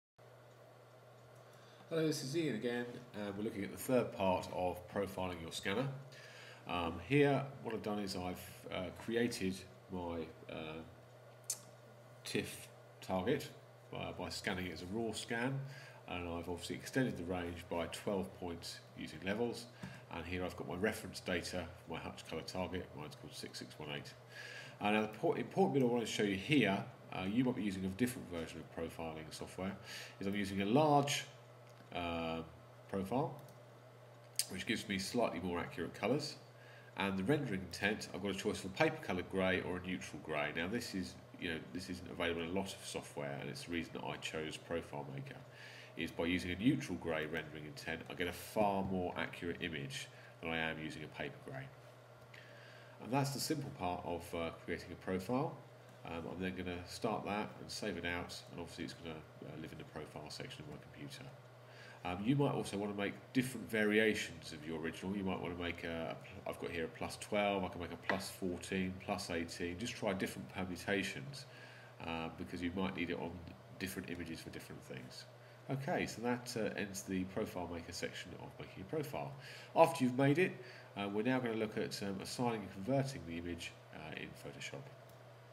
hasselblad scanner profiling using profilemaker
Making the profile in profilemaker, note the settings used and the rendering intent